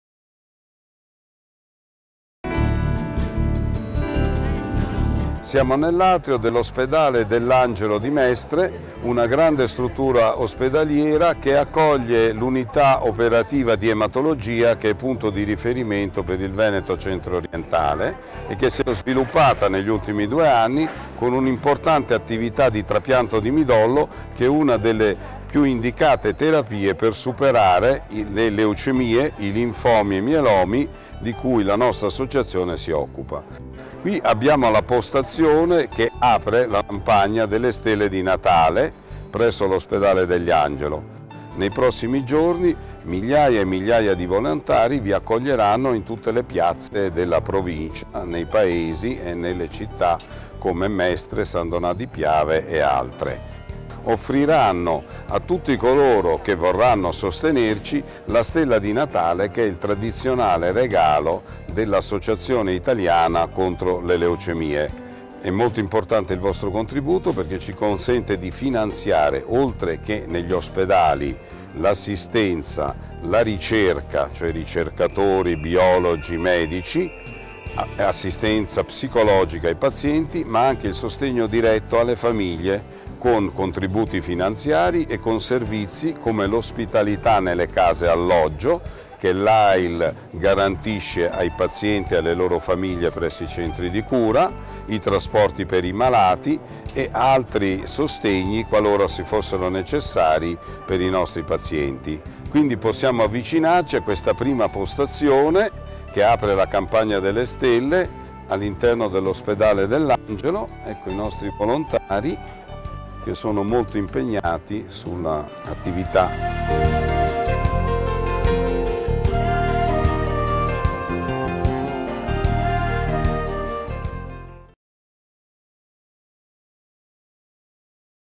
Ail, 25 anni di stelle
Dal 6 all'8 dicembre in 4mila piazze italiane 25° edizione delle Stelle di Natale dell'AIL, Associazione Italiana contro le Leucemie, Linfomi e Mieloma. Quest'anno c'è anche l'invito a mettere la stella nel proprio profilo whatsapp.
Appuntamento a Mestre all'ospedale dell'Angelo.
Il presidente dell'AIL Venezia, Giovanni Alliata di Montereale.